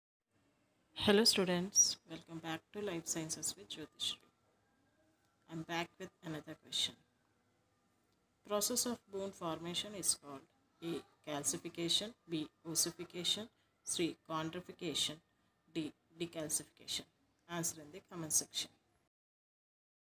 BONE Formation#Process#Anatomy and Physiology#Bones#Joints#yt shorts#shorts#
The natural process of bone formation.the hardening (as of muscular tissue) into a bony substances .a mass or particle of ossified tissue tendency toward or state of being molded into a rigid, conventional, sterile, or unimaginative condition